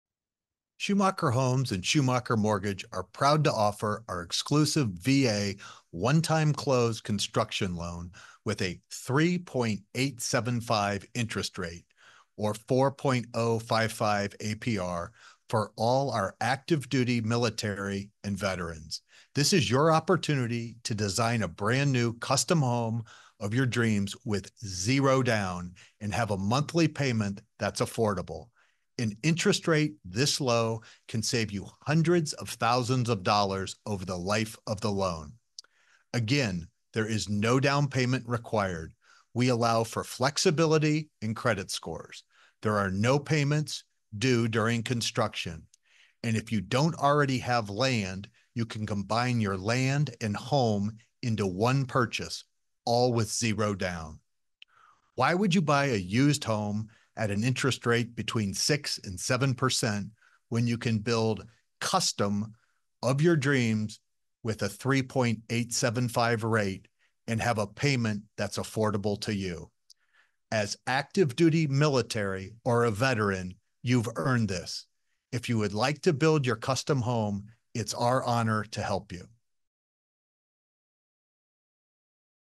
VA Construction Loans as low as 3.875% - 30 year fixed | Schumacher Homes
Hear from Paul Schumacher on 3.875% / 4.055% APR for our Active Duty and Military Veterans ! Don't miss out on this unique opportunity for zero down and no payments during construction. Take action today to lock in this rate and save thousands!